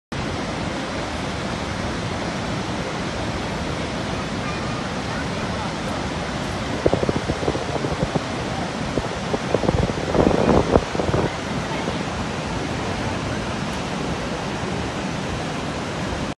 Niagara Falls ~2
Niagara Falls is a group of three waterfalls at the southern end of Niagara Gorge, between the Canadian province of Ontario and the US state of New York. From largest to smallest, they are Horseshoe Falls (also known as Canadian Falls), American Falls and Bridal Veil Falls. Horseshoe Falls straddle the international border between Canadaand the United States, while American Falls and Bridal Veil Falls lie entirely within the United States. Bridal Veil Falls are separated from Horseshoe Falls by Goat Island and from American Falls by Luna Island.